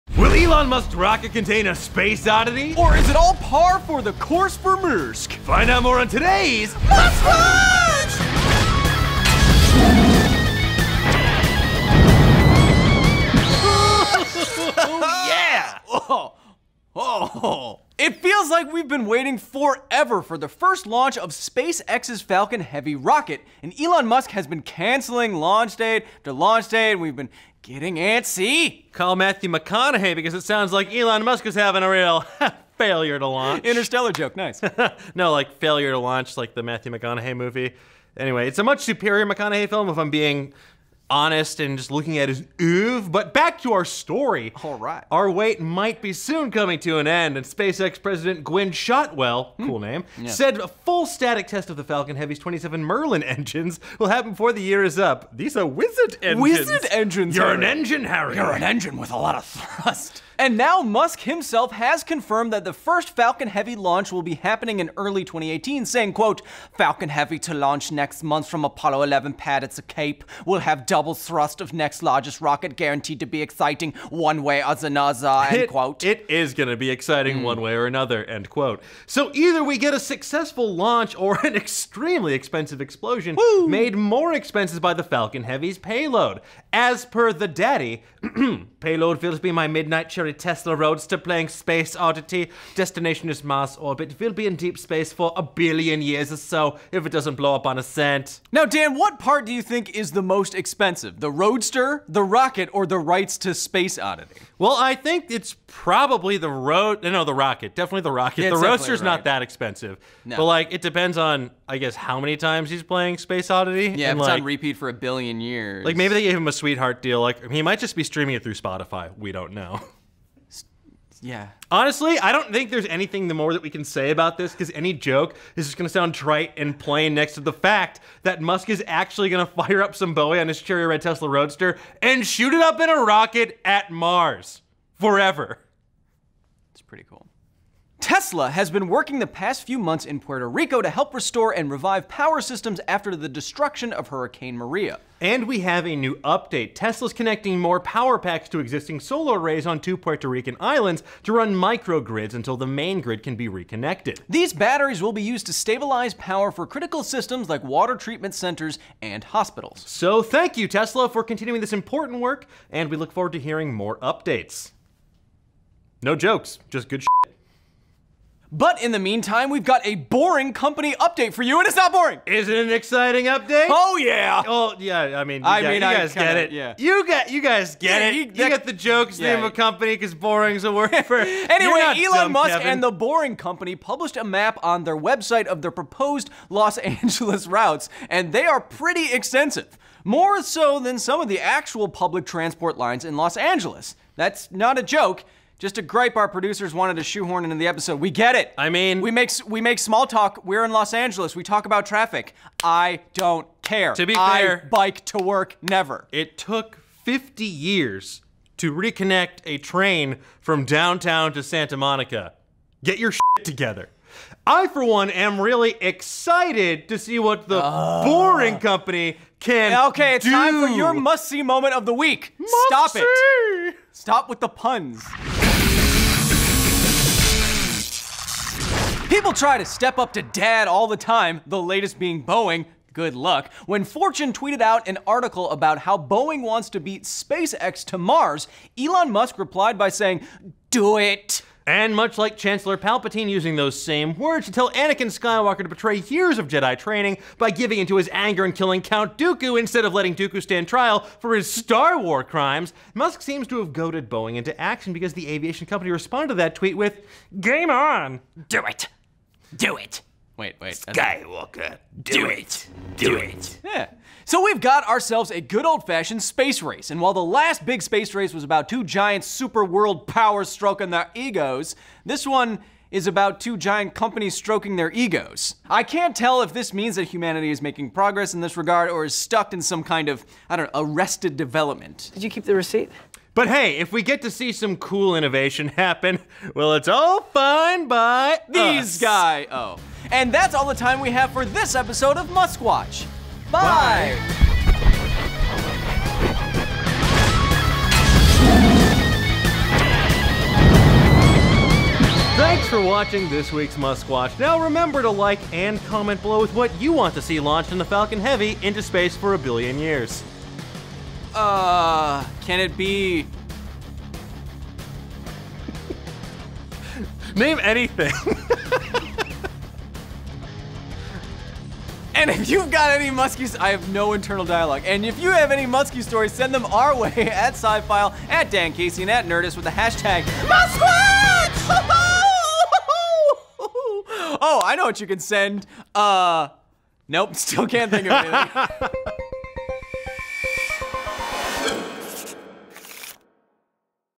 What Is Elon Musk Launching to Mars First? (Muskwatch w/ Kyle Hill & Dan Casey)
Elon's SpaceX is ready to send their Falcon Heavy to Mars, but what will it be carrying? Kyle and Dan discuss this and more on this week's Muskwatch!